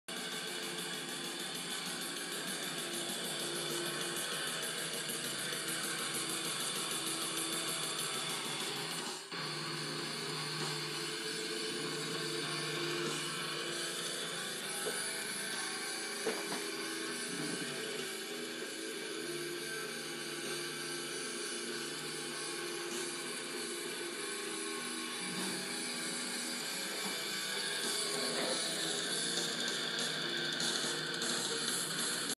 New song I'm working on
This is a sneak peak of a new song I'm working on. I also got a new midi keyboard as well.